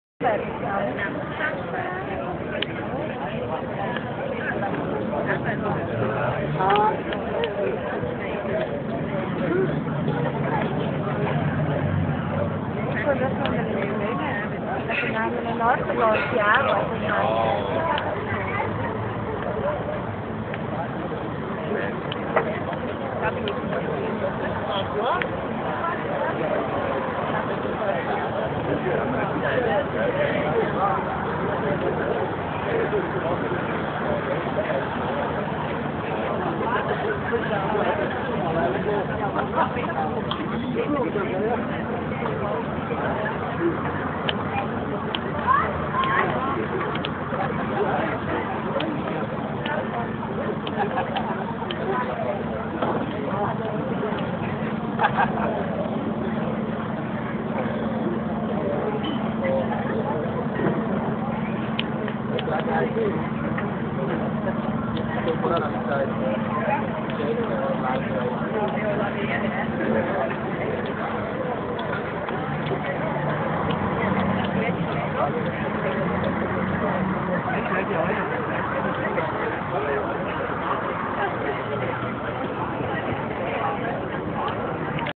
VASCO LIVE @ ZURIGO SA.08.05.2010 (1)
TANTI HANNO ASPETTATO PER ORE ... PRIMA DI POTER ENTRARE , DOVE CERANO TANTISSIMI DIVIETI , DI DIVERTIRSI , NO SMOKING ETC - IL PROIBITISMO CONTEMPORANEO HANNO UN PO ANNOIATO ... L'AGRESSIVITA DI UN DURO ROCK-ELETRIZZANTE , IN COMPENSO LA PROFONDITA ARTISTICA DELLA POESIA-POESIA HA DOMINATO :-) - FUEGO ASTRAL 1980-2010 LIVE @ DOCUMENT OF RADIO , TV SHOW, VIDEOS AND MUSIC MIXES